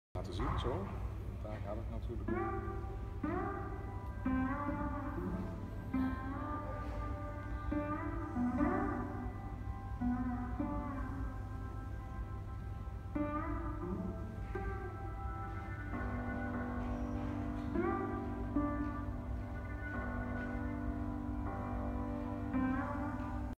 Hoe werkt de CRDL?
PIDZ ging op bezoek bij de makers van de CRDL, een instrument dat mensen weer tot elkaar brengt.